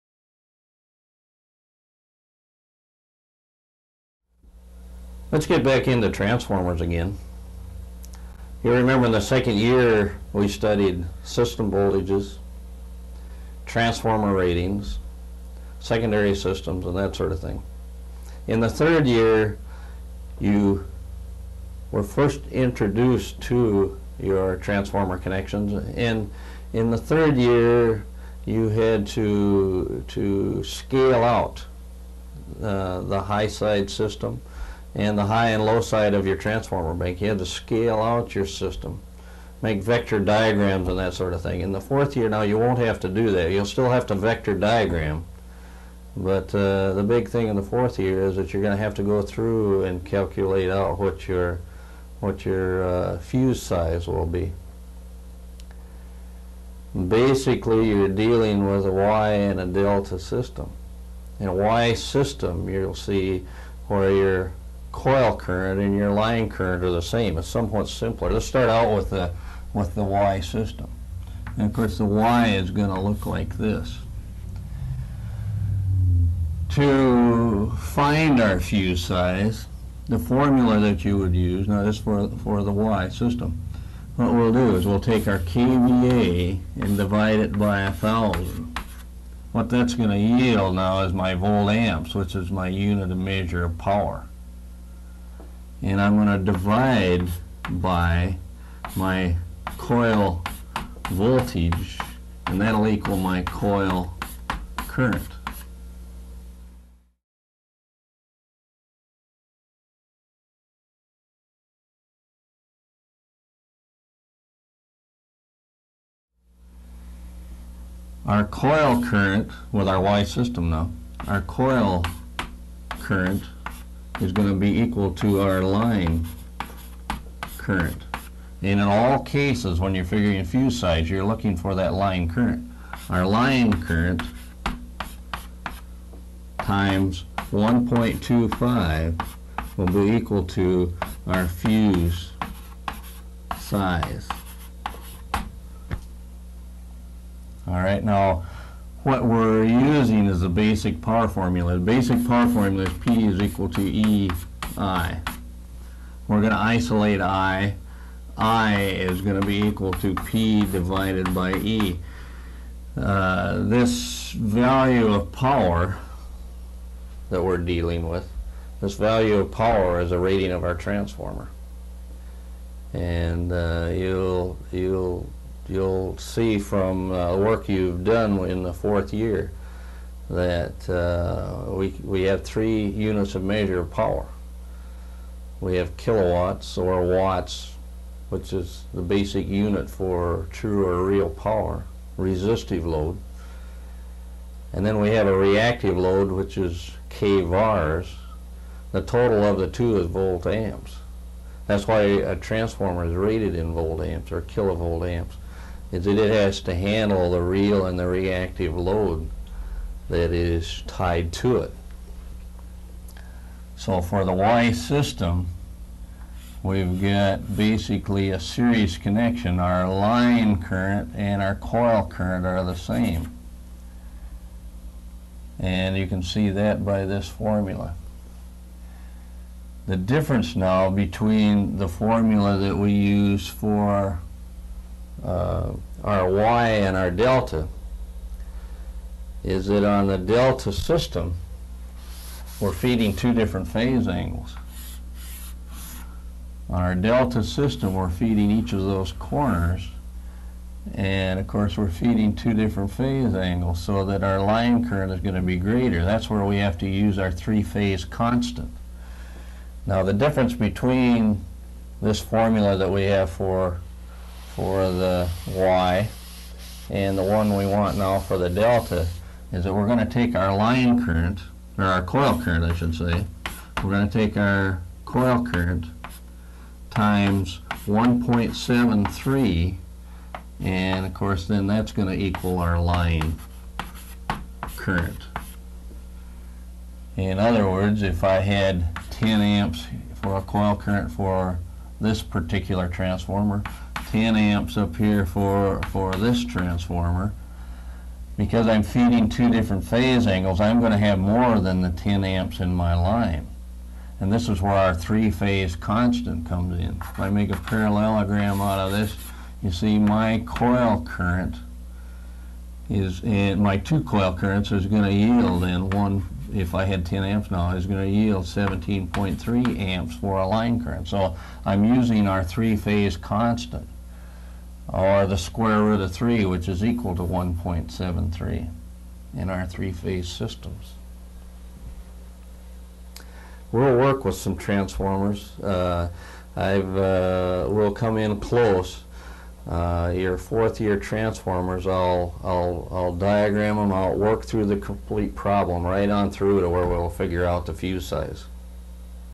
Transformer Fusing
Dennis Merchant video on calculating transformer coil current and line current for sizing fuses.  This video goes with the 4th year transformer connections portion of the Merchant Job Training & safety Program.